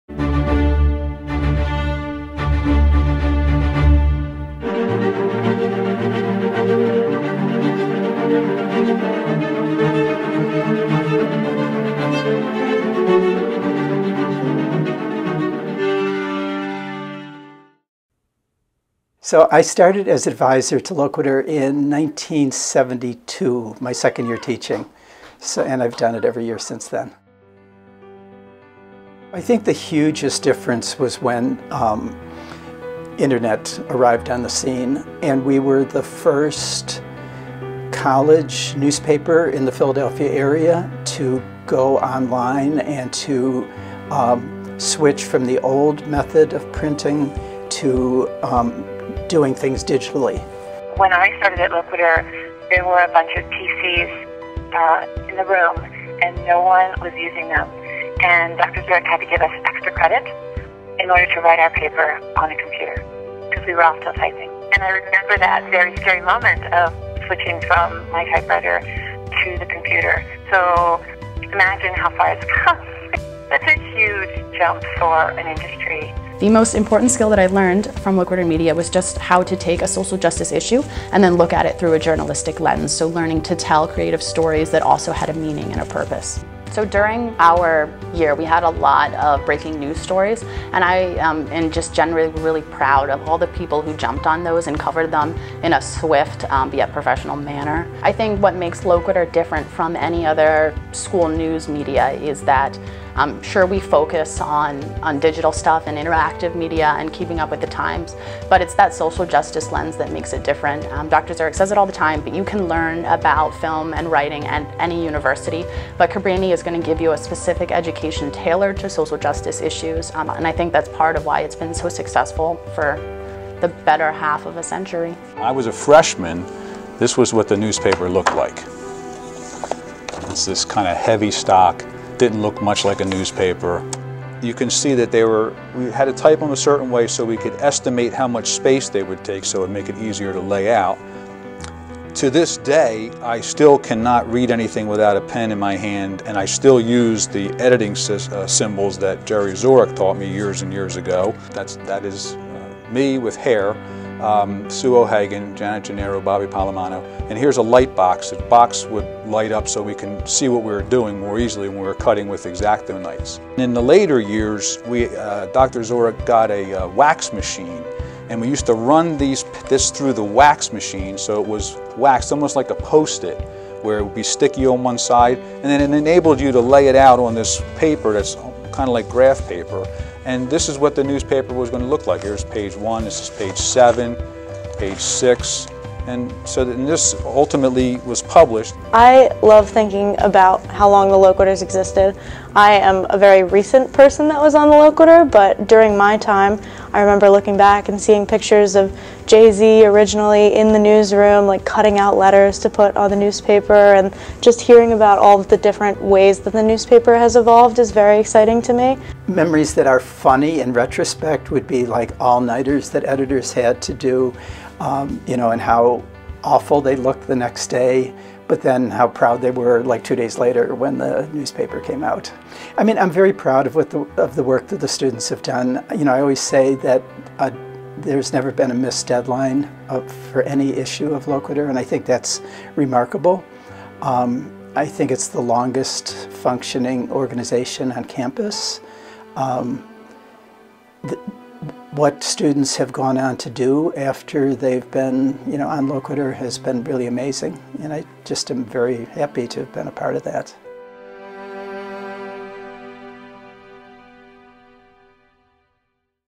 Loquitur celebrates 60 years
Loquitur alumni reflect on their time working for the paper.